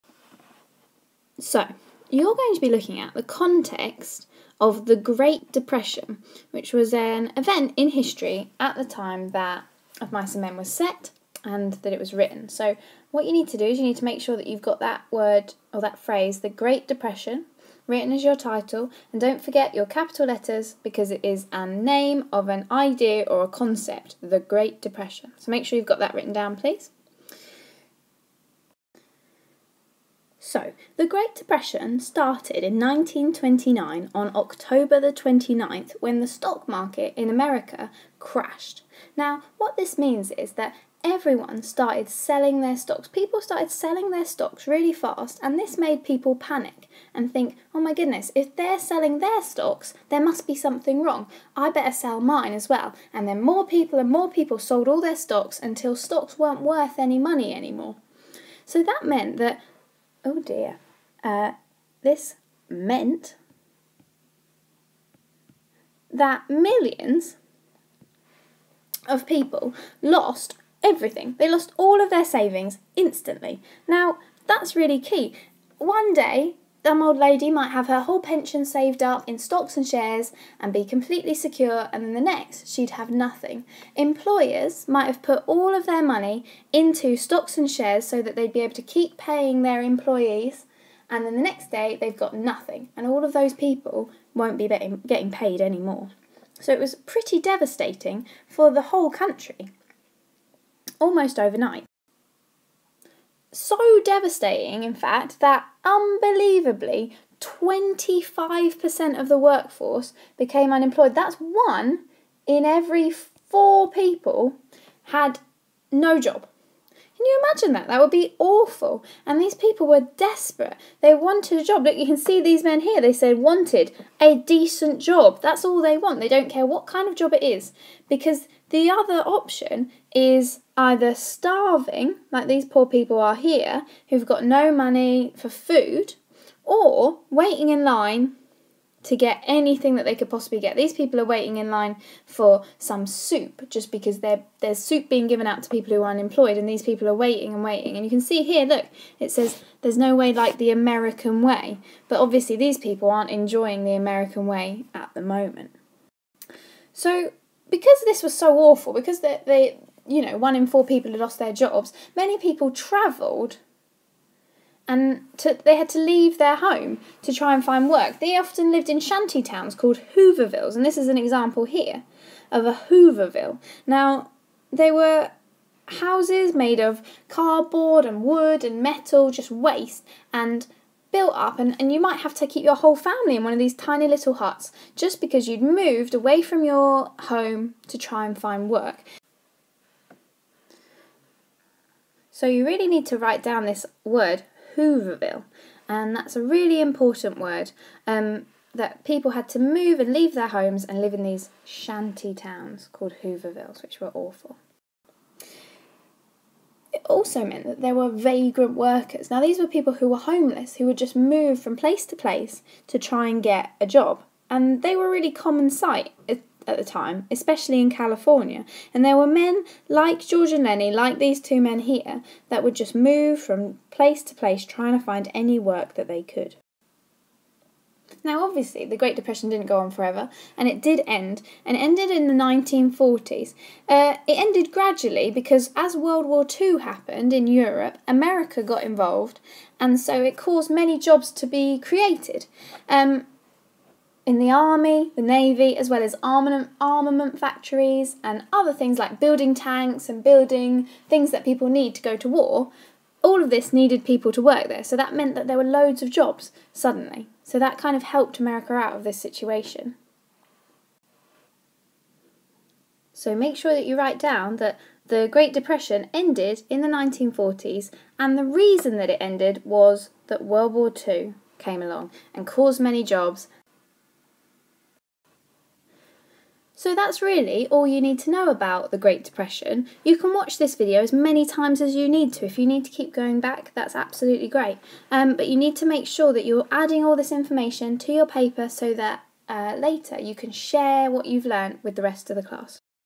Of Mice and Men's Context: The Great Depression
Basic information on the Great Depression's relevance to John Steinbeck's Of Mice and Men.